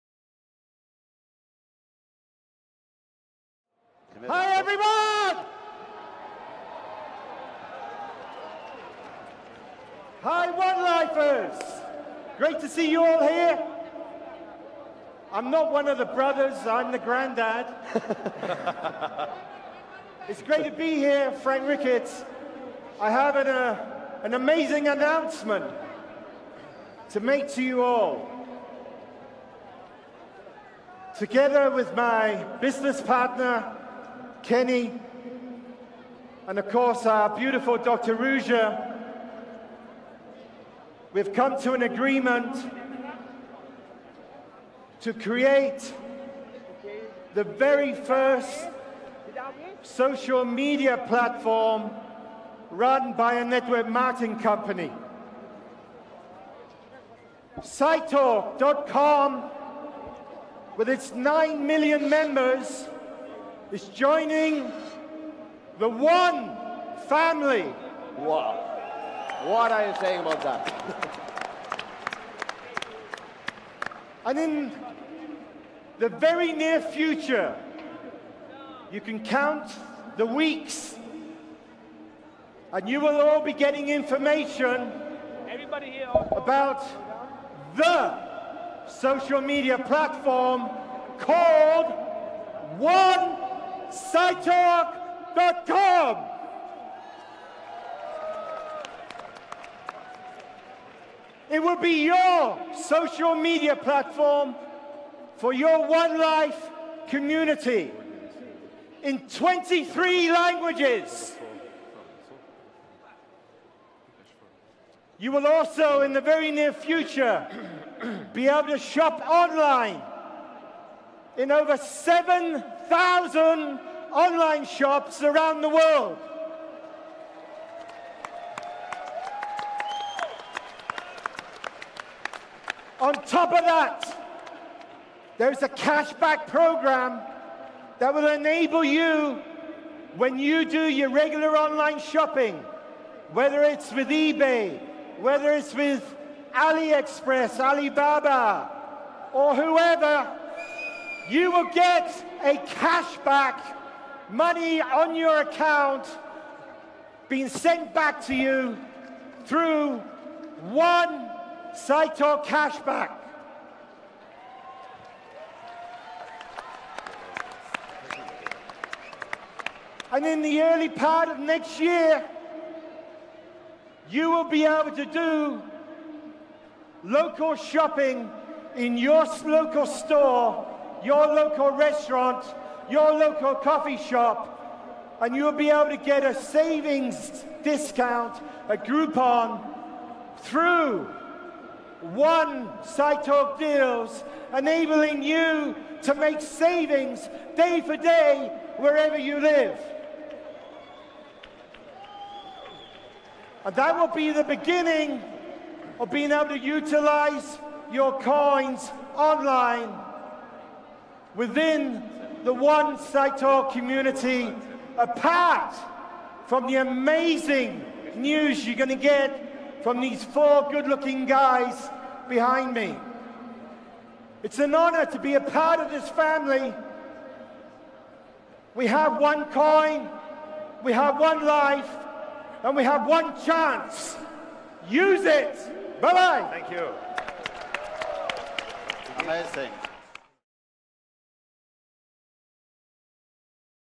SideTalk Joins with OneLife OneCoin Family.
It is now called… Onesitetalk
With all its shop including Alibaba, Ebay and over 7000 other online shops will accept Our Crypto Currency Coins.